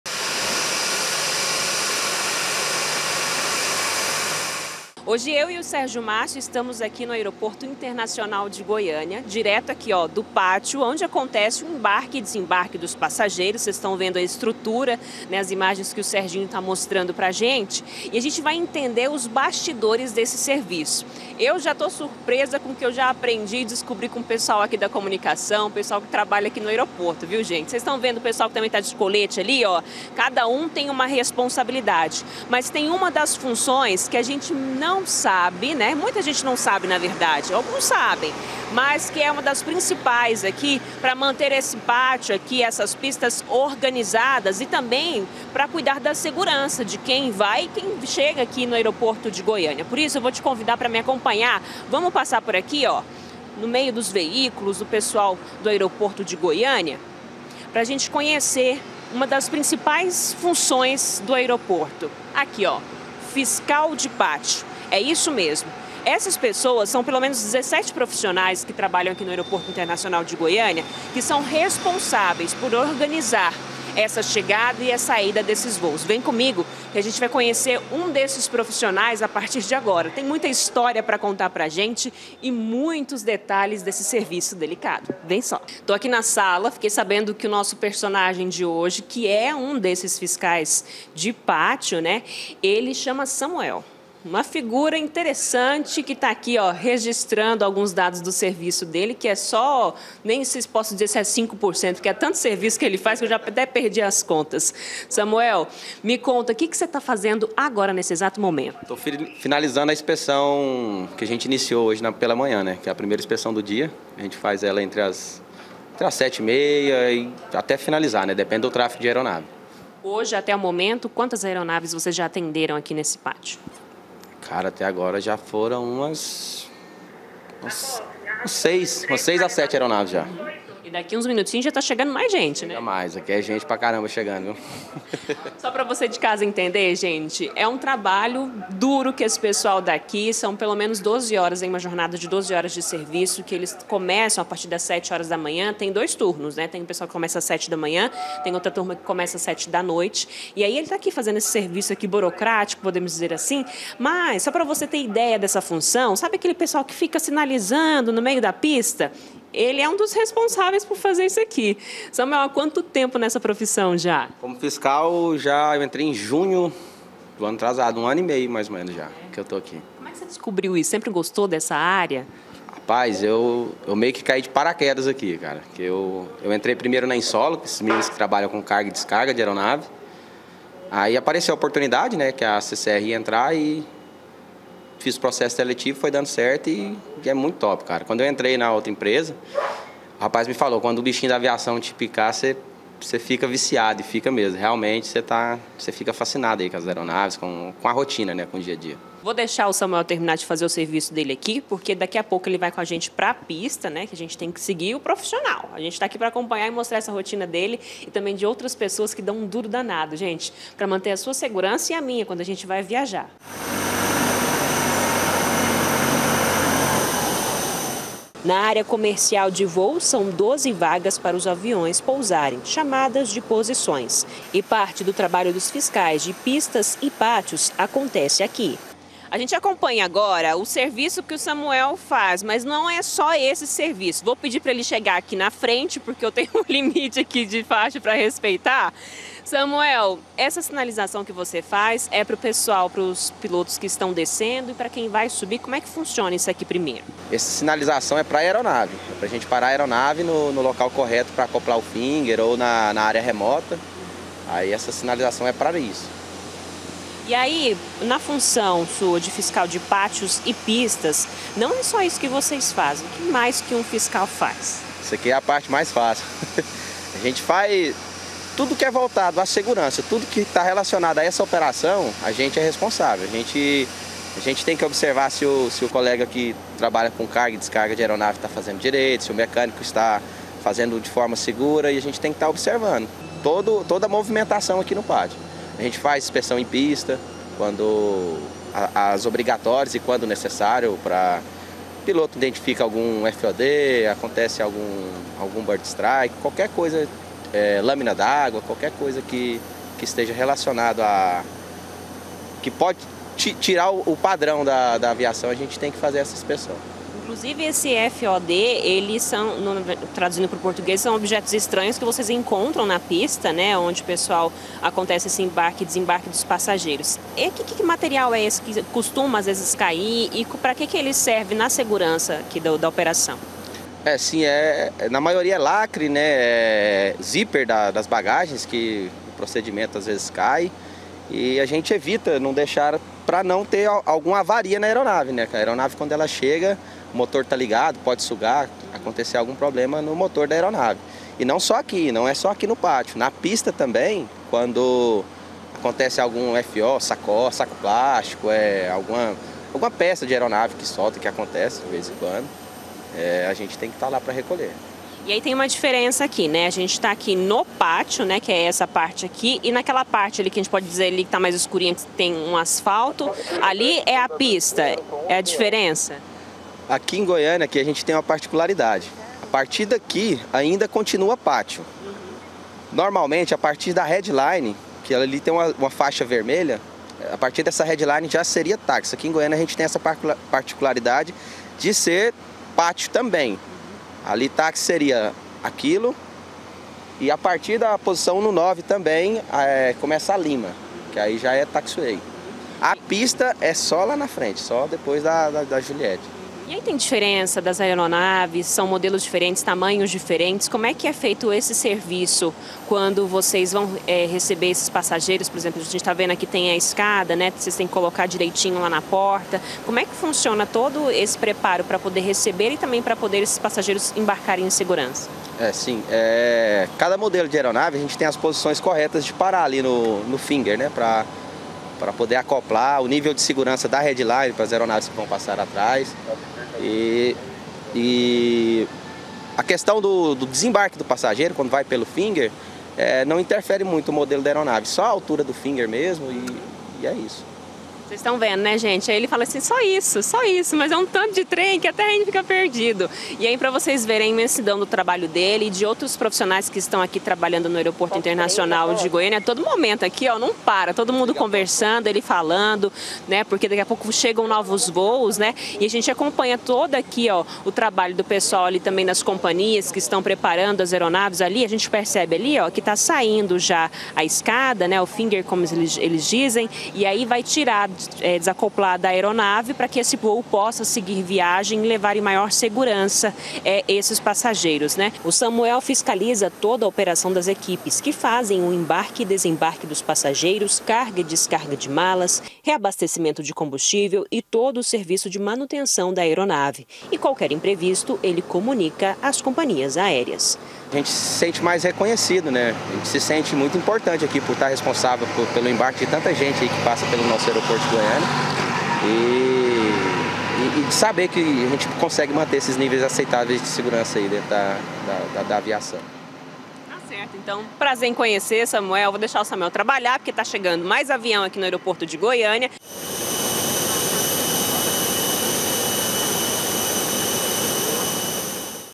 FISCAL E PÁTIO: O RESPONSÁVEL PELA ENTRADA E SAÍDA DE AERONAVES
HOJE A NOSSA EQUIPE VAI TE CONTAR SOBRE UMA DAS PROFISSÕES MAIS IMPORTANTES PARA MANTER A SEGURANÇA DAS OPERAÇÕES AEROVIÁRIAS. VOCÊ JÁ DEVE TER VISTO UM FISCAL DE PÁTIOS E PISTAS NO AEROPORTO, MAS NÃO SABE DOS DETALHES QUE ESSA GALERA DESEMPENHA. A LARISSA DOURADO E O SÉRGIO MÁRCIO FORAM ATÉ O AEROPORTO INTERNACIONAL DE GOIÂNIA E MOSTRAM TUDO PRA GENTE.